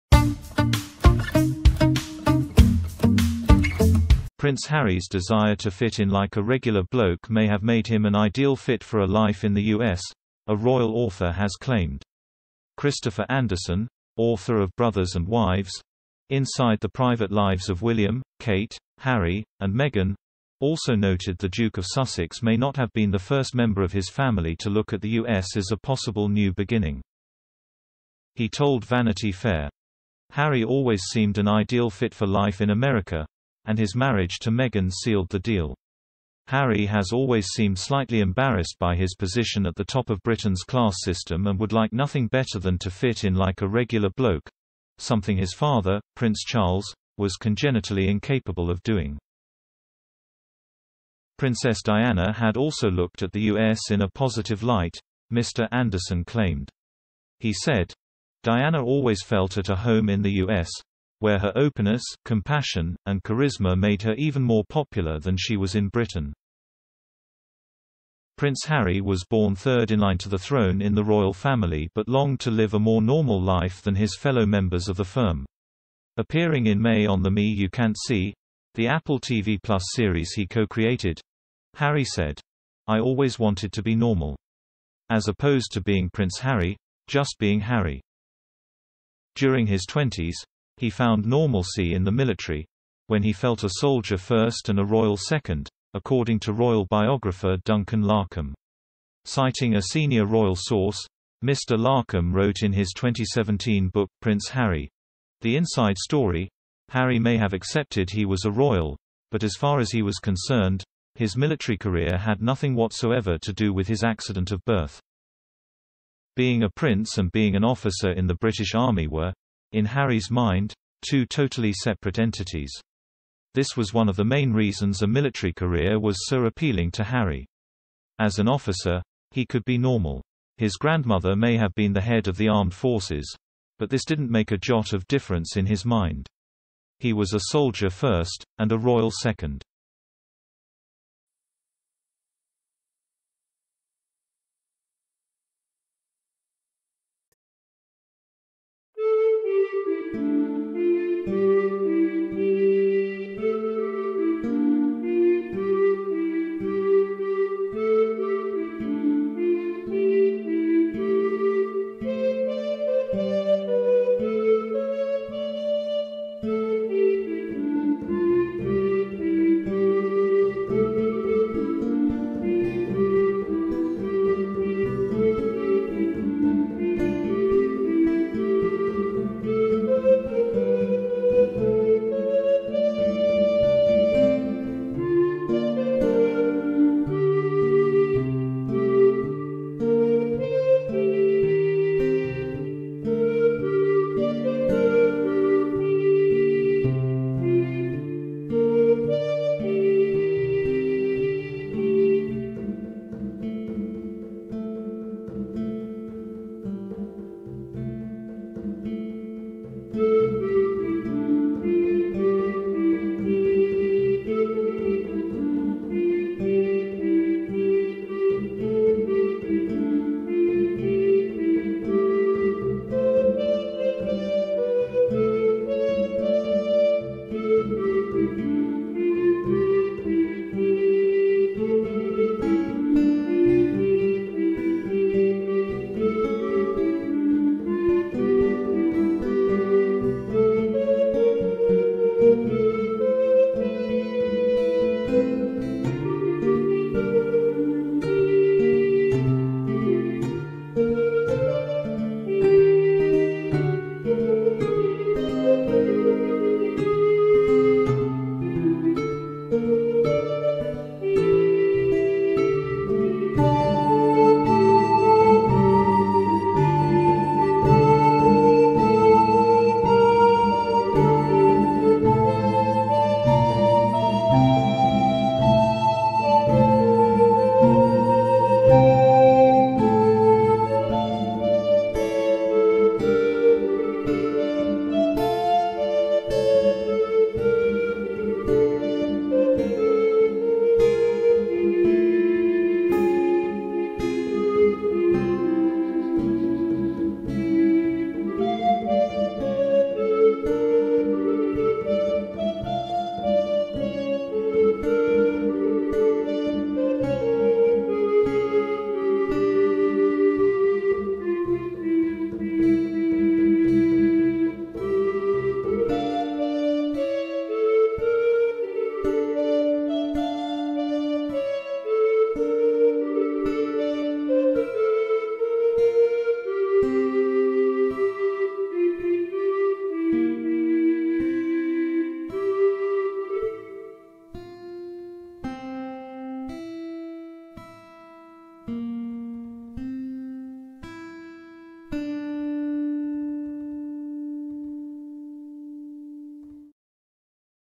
'Ideal for life in US' Prince Harry was 'embarrassed' by his privileged position in UK
Prince Harry's desire to "fit in like a regular bloke" may have made him an "ideal fit" for a life in the US, a royal author has claimed.

Christopher Andersen, author of 'Brothers and Wives: Inside the Private Lives of William, Kate, Harry, and Meghan', also noted the Duke of Sussex may not have been the first member of his family to look at the US as a possible new beginning.

He told Vanity Fair: "Harry always seemed an ideal fit for life in America, and his marriage to Meghan sealed the deal.

"Harry has always seemed slightly embarrassed by his position at the top of Britain's class system and would like nothing better than to fit in like a regular bloke — something his father, Prince Charles, was congenitally incapable of doing."

Princess Diana had also looked at the US in a positive light, Mr Andersen claimed.

He said: "Diana always felt at a home in the US, where her openness, compassion, and charisma made her even more popular than she was in Britain."